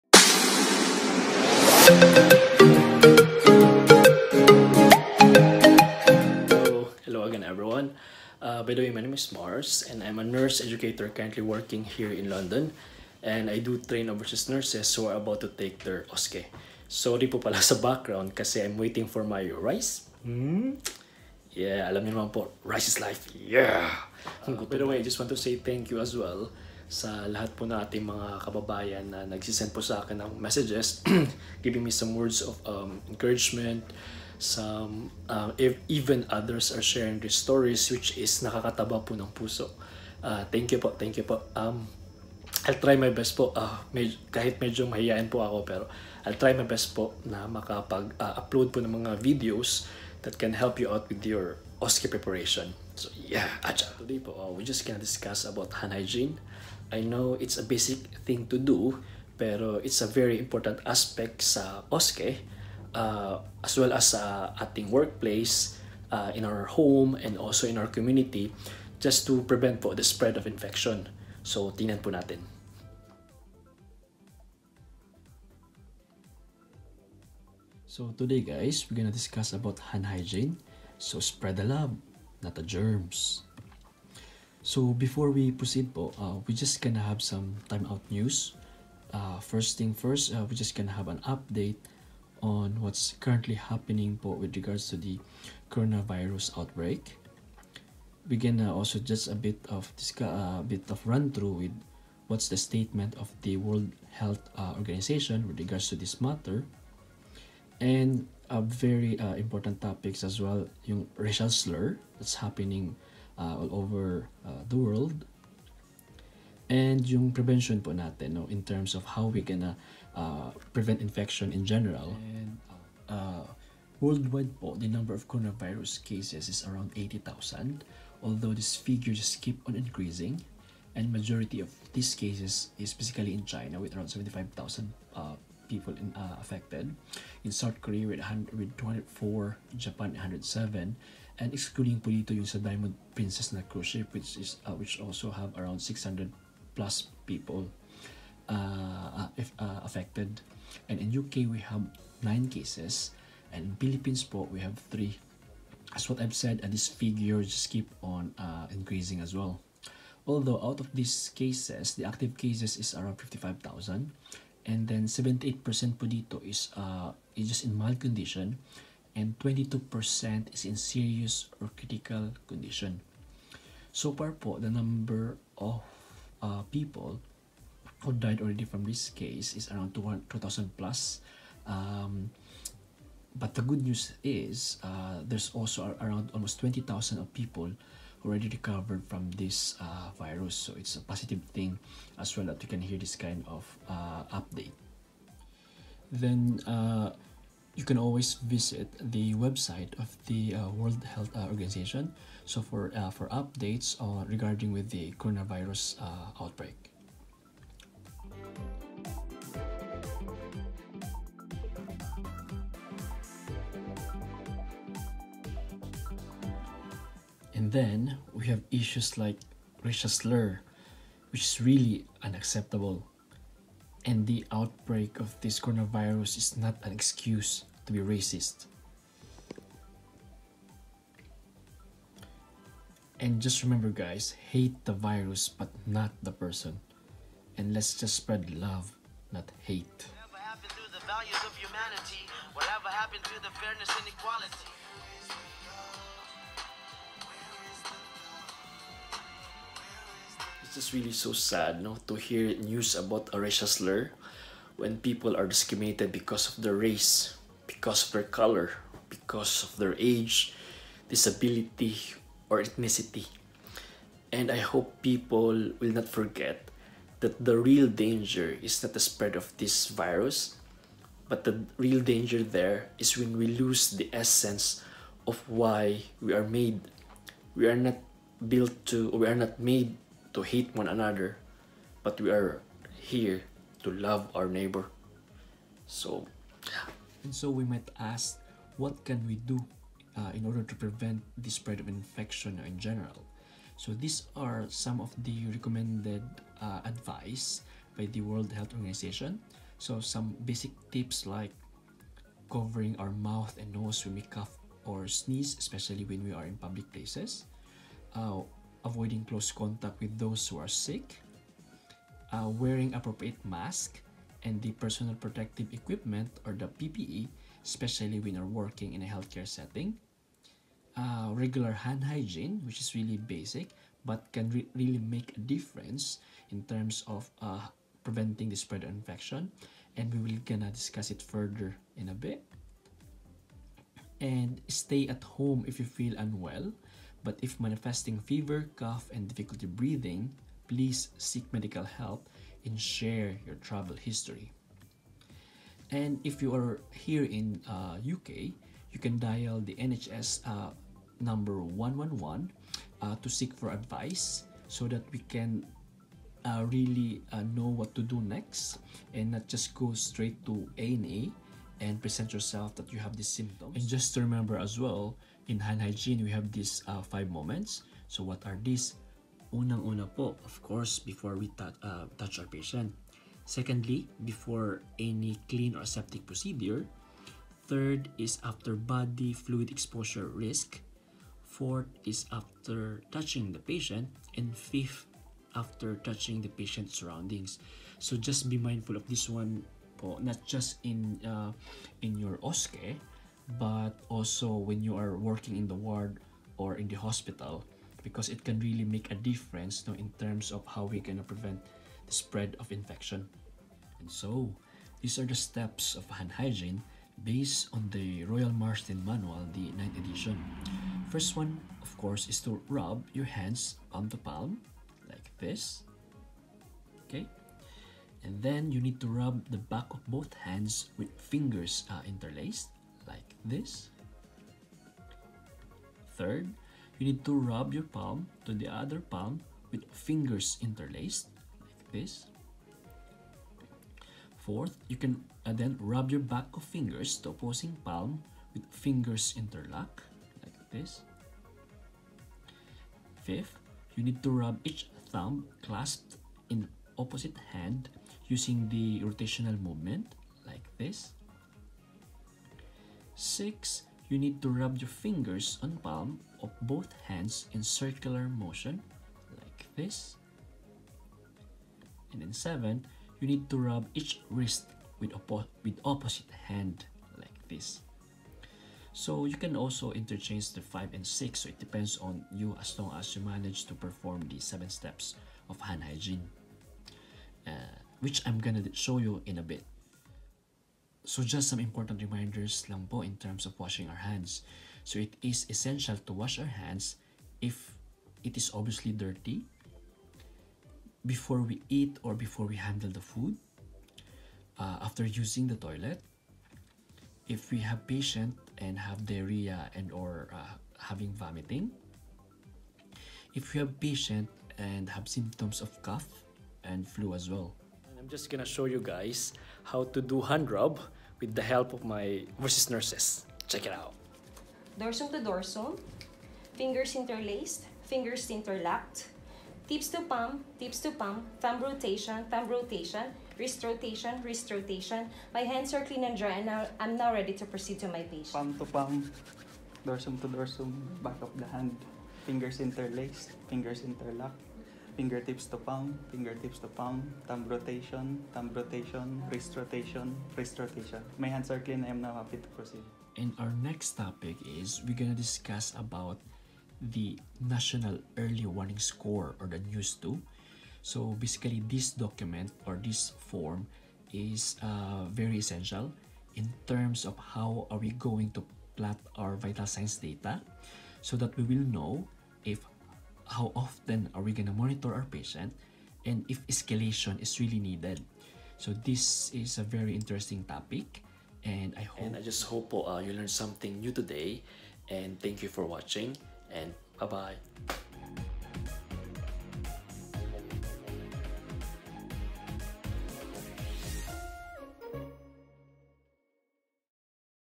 Basic Hand Hygiene Technique - the OSCE way
Please visit the World Health Organisation Website for more updates on CoronaVirus outbreak.

Hand hygiene - a basic thing to master in OSCE.